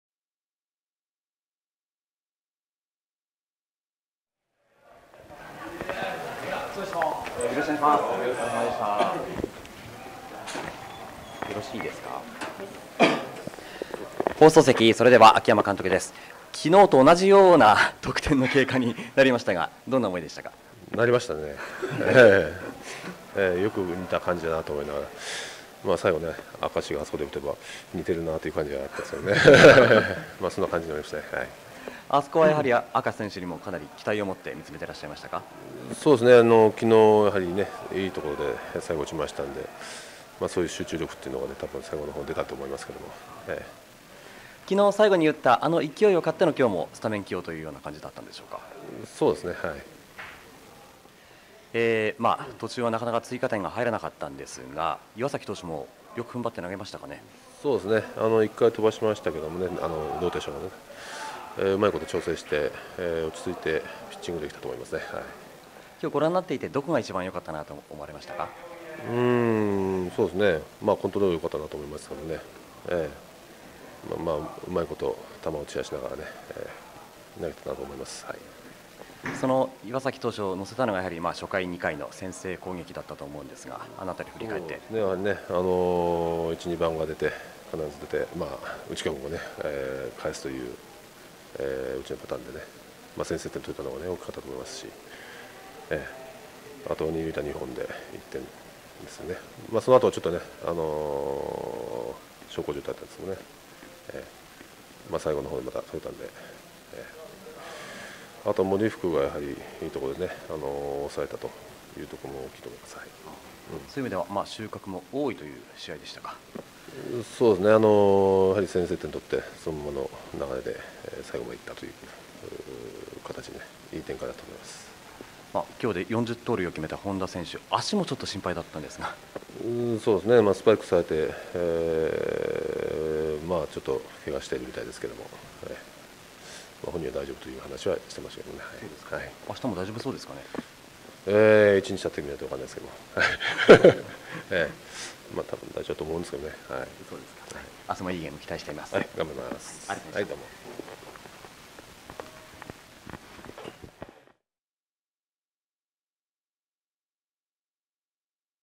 ソフトバンクホークス 秋山幸二 監督インタビュー 20110819
2011年8月19日[ヤフードーム]H 4ｰ1 Eでホークスが勝利しました。試合後の秋山監督インタビューをご覧ください。※音声が一部聞き取りにくい場面がございます、ご了承ください。